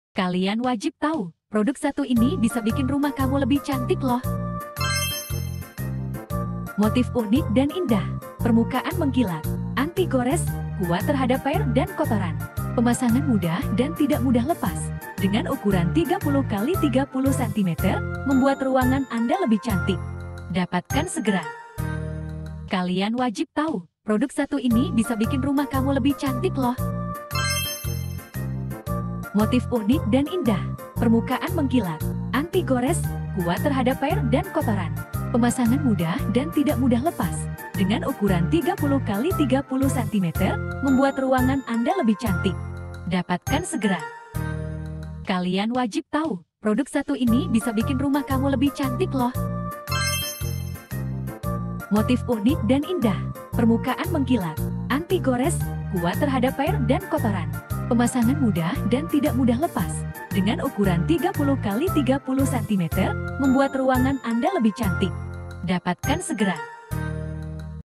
🌰 Kehangatan Alam: Zehn Stein Mosaic GS-07 Exotic Brown Sugar 30x30cm 🌟
Hadirkannya kehangatan alam ke dalam rumahmu dengan Zehn Stein Mosaic GS-07 Exotic Brown Sugar! 🌰 Dengan motif yang terinspirasi oleh kekayaan alam, mosaik ini membawa nuansa hangat dan menyenangkan ke dalam ruangan.

Ideal untuk dinding, lantai, atau elemen dekoratif lainnya, mosaik ini akan memberikan sentuhan alami yang memikat dan menenangkan. Dengan ukuran 30x30cm, instalasinya mudah dan memberikan fleksibilitas dalam desain.

Dapatkan sekarang dan hadirkan kehangatan alami ke dalam rumahmu dengan Zehn Stein Mosaic GS-07 Exotic Brown Sugar! 🌟